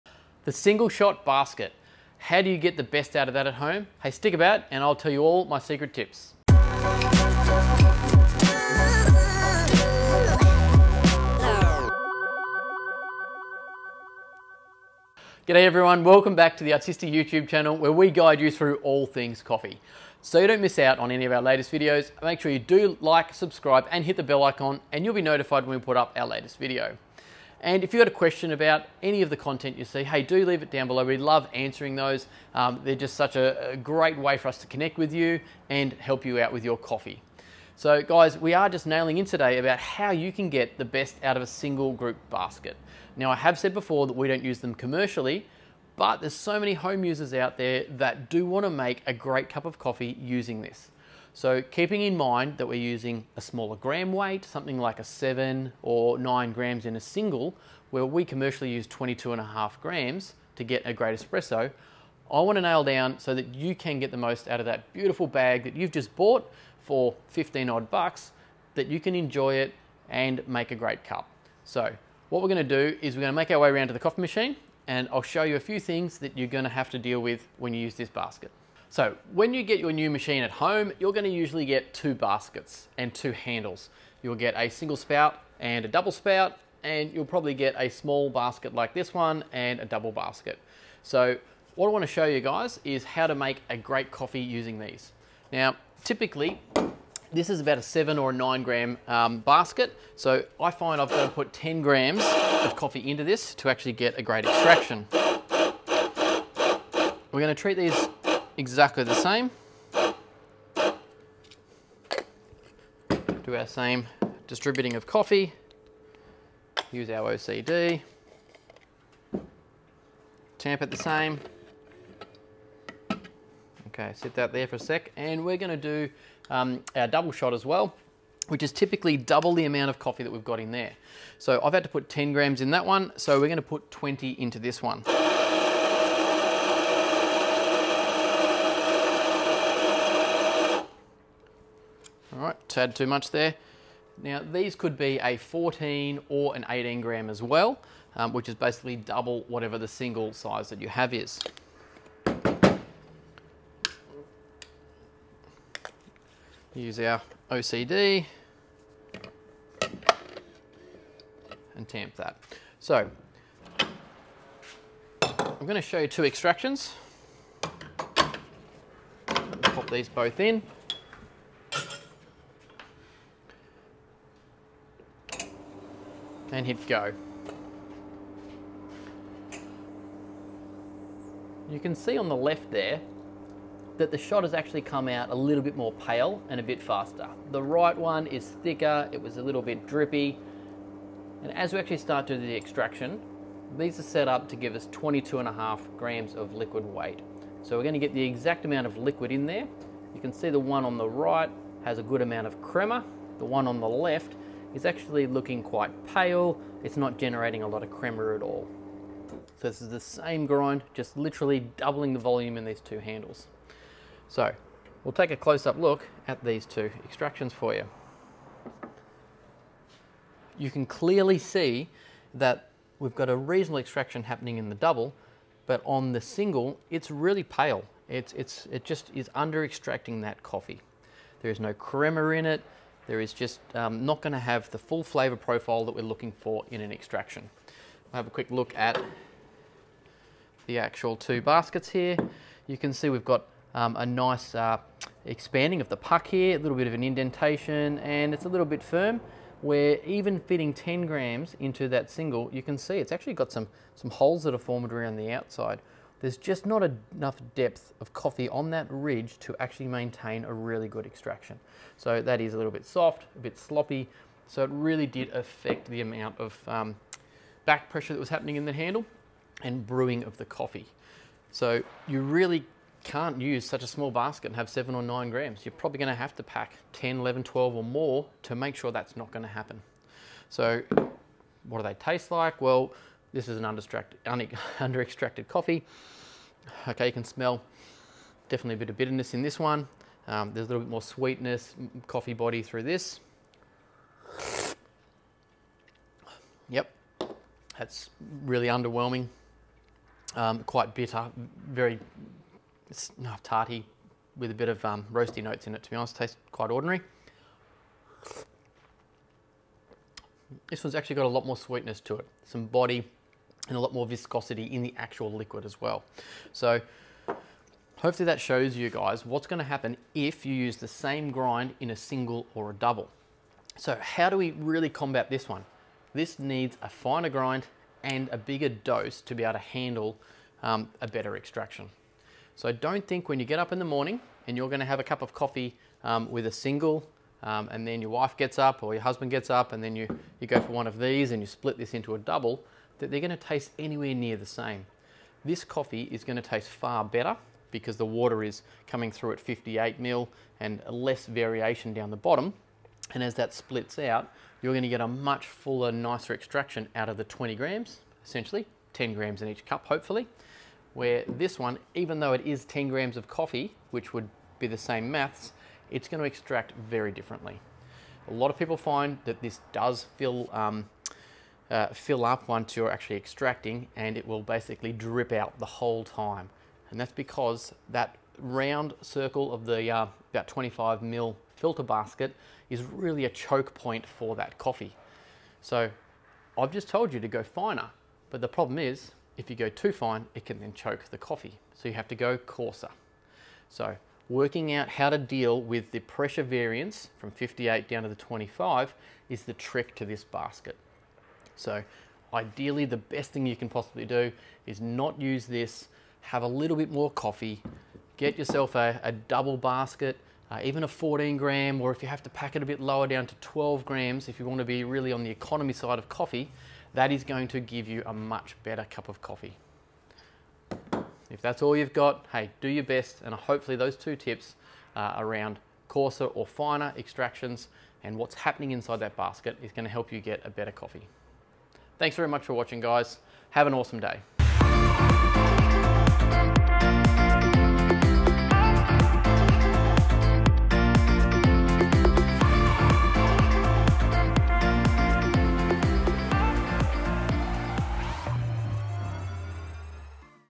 Single Spout Handle with 7 - 9 gram Basket - What you need to know.
If you're using a single spout single basket handle at home and your struggling to get decent results well perhaps this video is for you. Here Luke talks you through some of his key tips to extracting good coffee when using the single handle option.